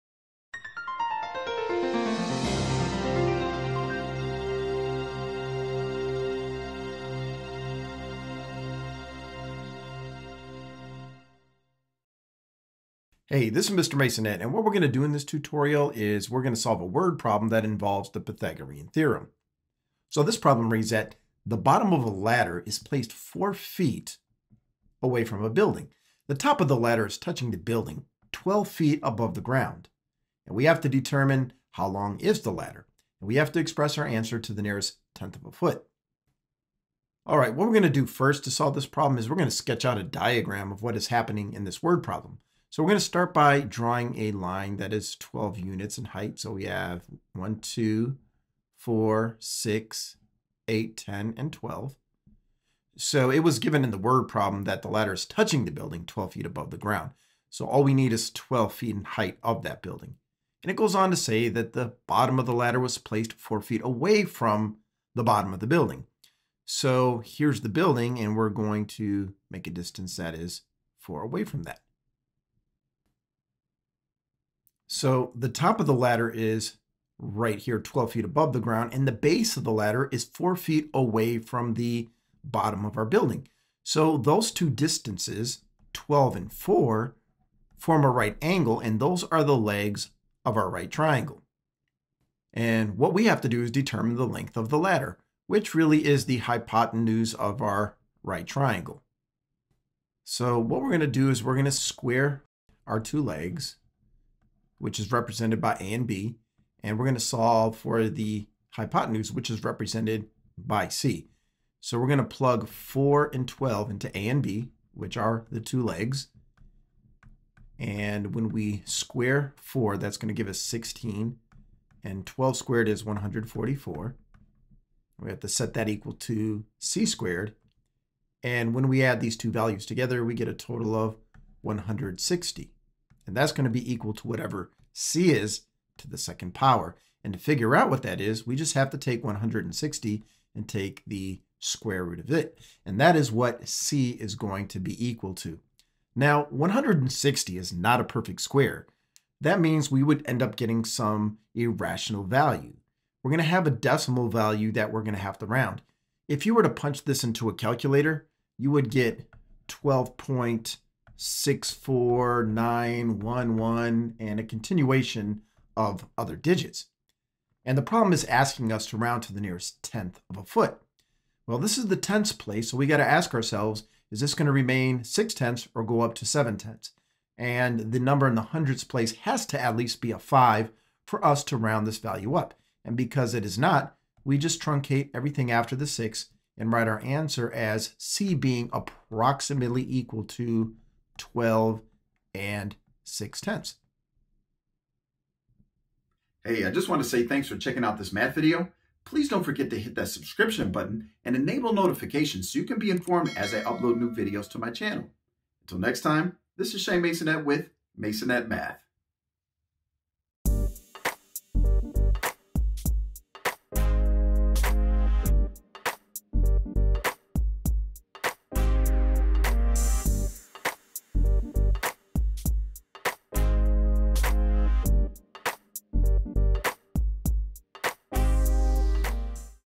The Pythagorean Theorem Word Problems
This video shows how to find the length of a ladder leaning against a building using the Pythagorean Theorem.